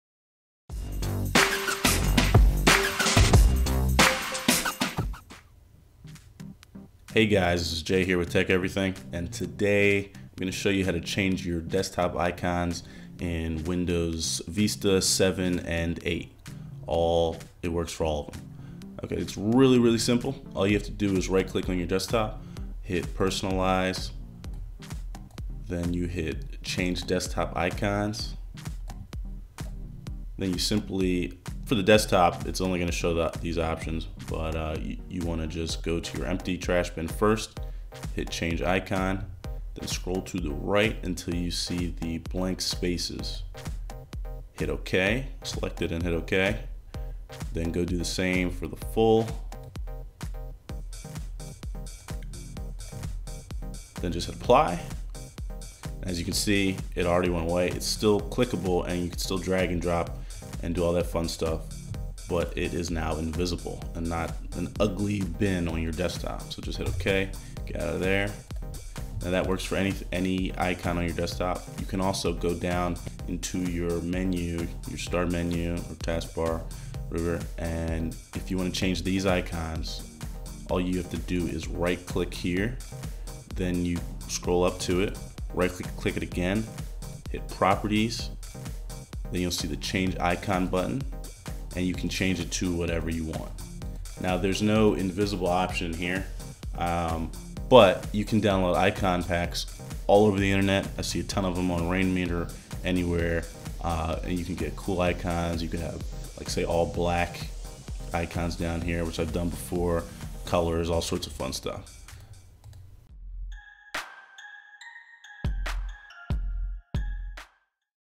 How to Change Your Desktop and Taksbar Icons (or make them invisible!)
Here is a quick guide on how to change your desktop and taskbar icons, or make them invisible